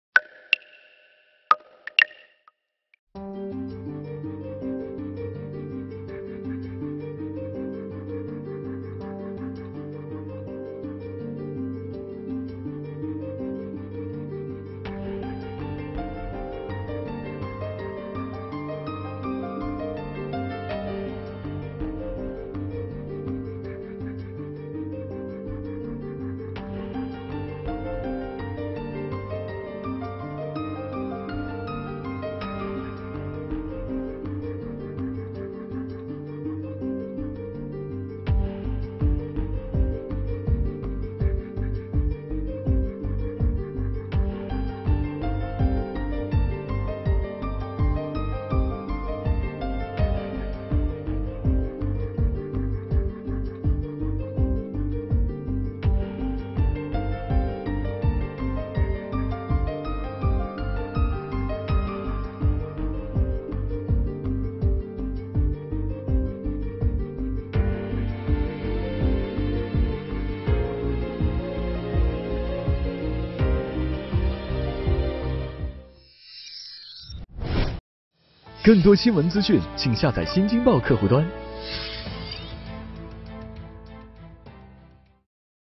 监控曝光！秘鲁酒店抢劫案1死2伤 大使馆：41名中国游客安全-新京报·动新闻
当地时间2月19日晚，秘鲁豪华酒店遭抢劫。事件造成1死2伤。当时酒店共有53名不同国籍游客。21日，中国使馆回应中美游客秘鲁被绑架称，酒店中共有41名中国籍游客，劫匪杀害一名秘鲁籍导游并抢夺财物后逃逸。截至目前没有中国籍公民在此次事件中伤亡，仅一人护照被抢。